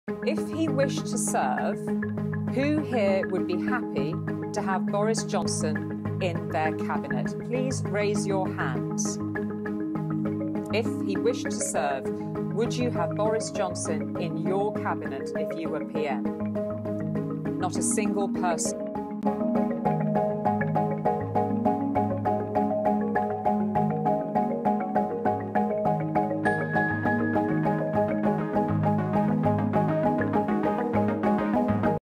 【場面尷尬：誰願意招攬約翰遜入閣？無人舉手】
7月18日，英國保守黨黨魁選戰，獲得晉級的五名參選人參加第二場電視辯論，圍繞稅務政策及通脹問題激烈交鋒。有民調顯示，前財政大臣蘇納克表現較佳，以超過四成的支持率，繼續領跑。蘇納克強調，他的優先工作是壓抑通脹，確保通脹不會惡化，才會考慮減稅。主持人問到，如果現任首相約翰遜想留在新政府，誰會願意招攬他入閣，現場無人舉手。